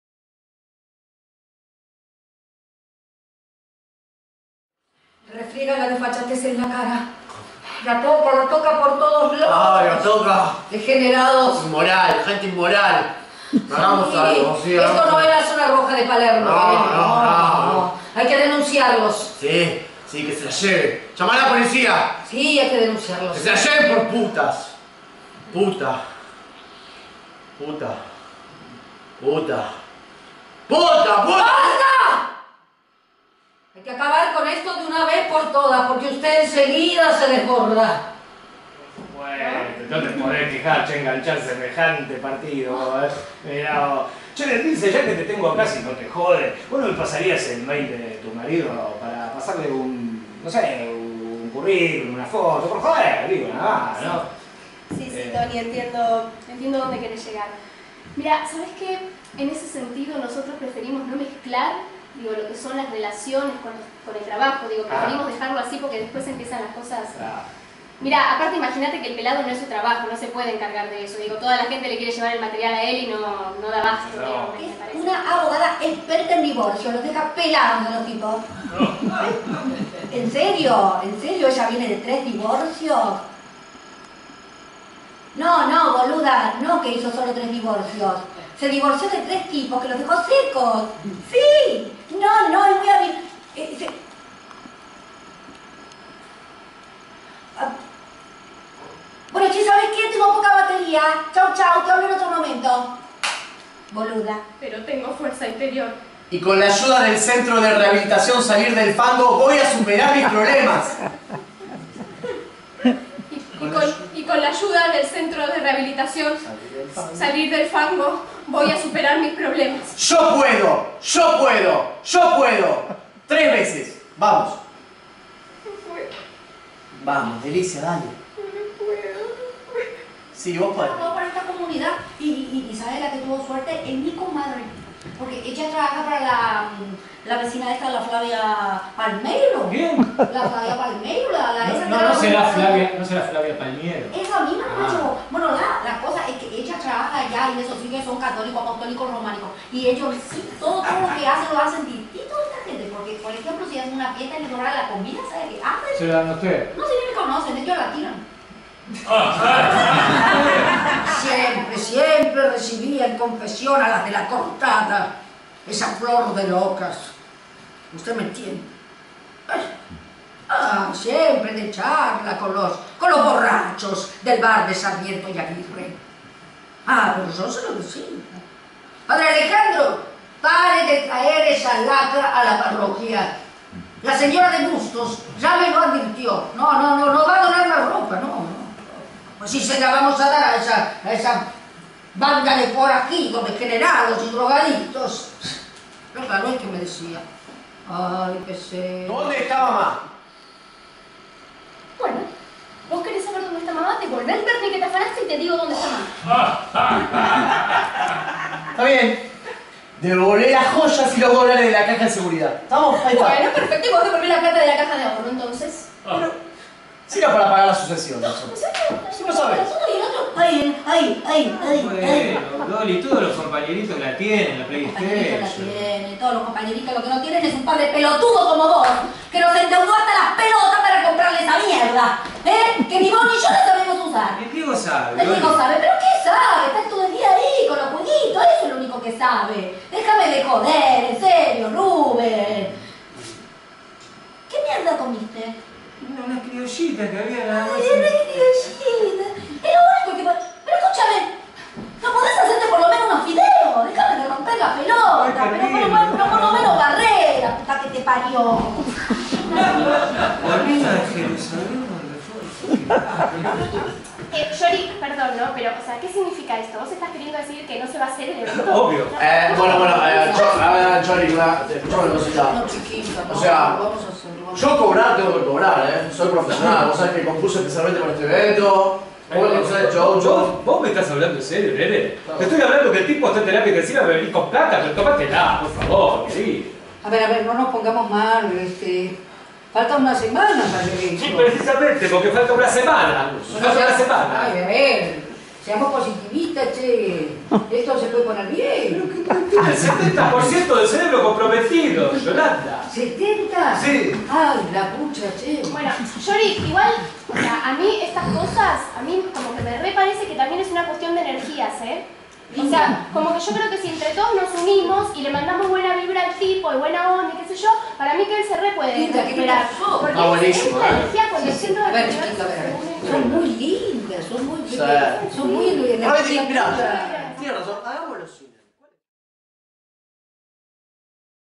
"CAZUELA DE PECADOS" de Pepe Bobe
Espectáculo estrenado en el teatro La Máscara
Dirección Guillermo Farisco